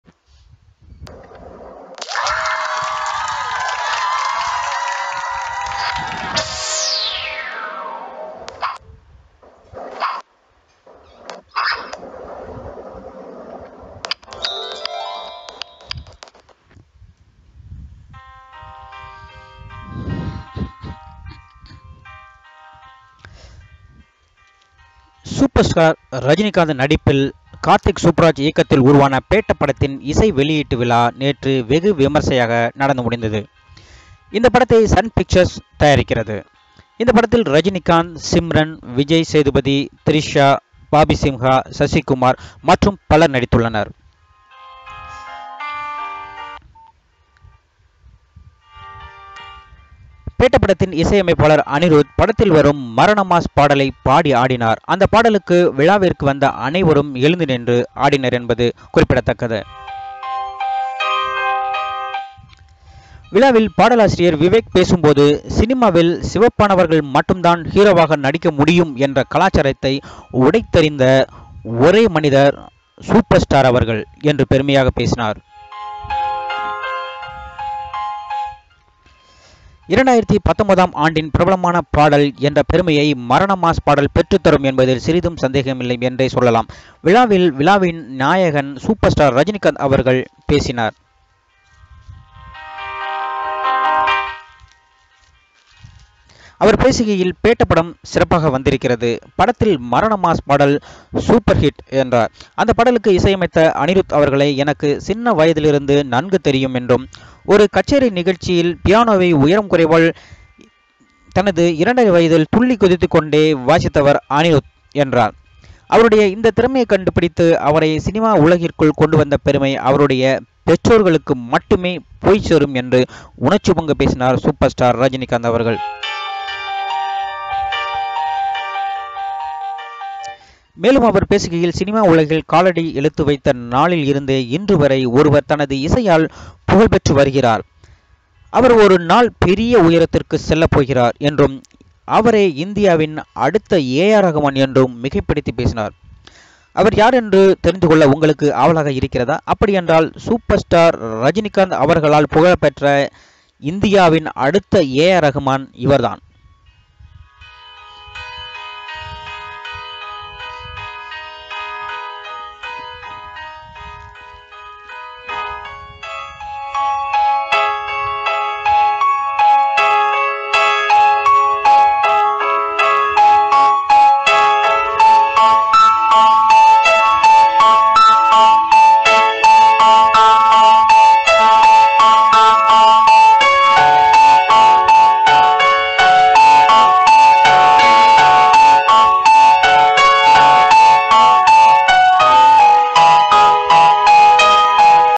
இந்தியாவின் அடுத்த ஏஆர் ரஹ்மான் இவர்தான் - பேட்ட இசைவெளியீட்டில் ரஜினி பேச்சு
Please watch this video which is about Super Star Rajinikanth's speech "He is next AR Rahman in India".